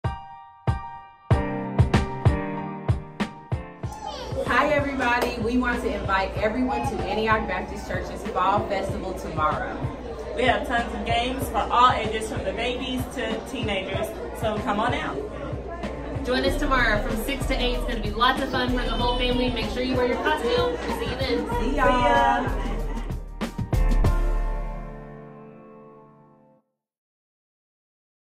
COME TO THE FALL FESTIVAL @ ANTIOCH BAPTIST CHURCH
Invite your friends and family to join us for the Fall Festival on Monday, October 31st from 6 p.m. to 8 p.m. There will be candy, pizza, hayrides, fun games & activities for the children. Children are encouraged to dress in family friendly costumes.